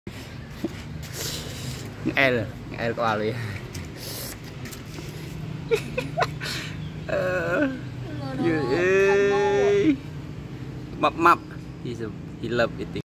Cute stray puppy in monastery
Lovely pagoda's puppy, James!! He loves eating food I give him.